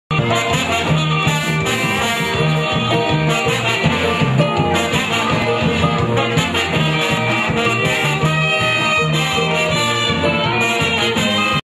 Introdução do vídeo Carnaval Heliodora 2010 - Segunda-f eira
Desfile de Blocos (Segunda-feira)
Bloco de Plumas e Paetês;
Banda Energia Tropical;
Evento: Carnaval/Heliodora2010;
Local: Av.Alvarenga Peixoto;
Cidade: Heliodora-MG.